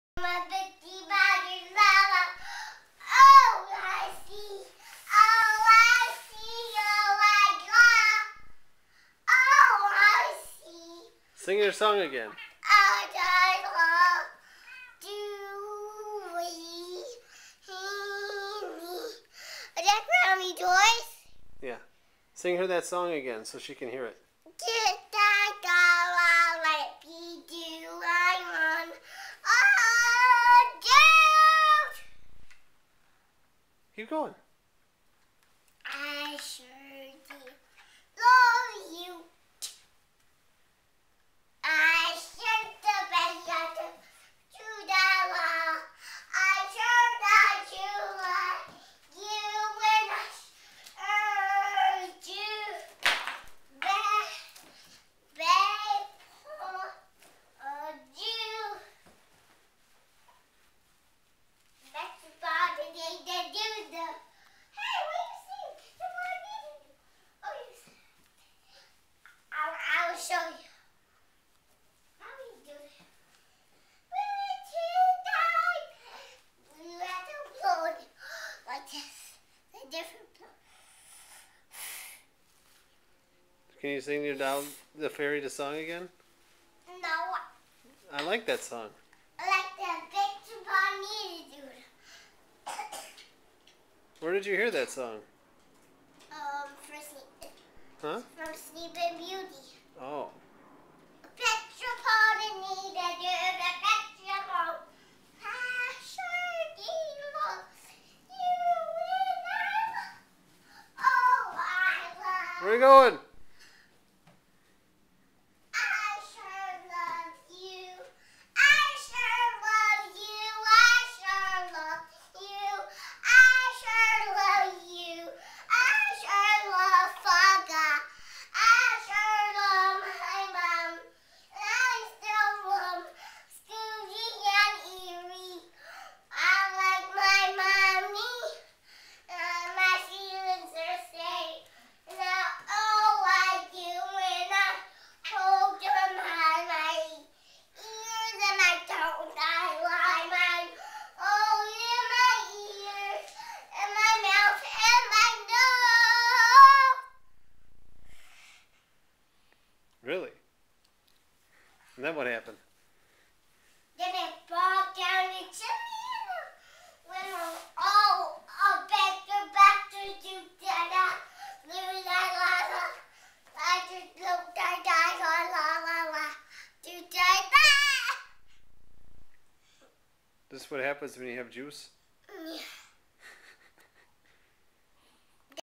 Kyleigh sings
Too much orange juice contributes to sugar rush for 2 1/2 year old Kyleigh.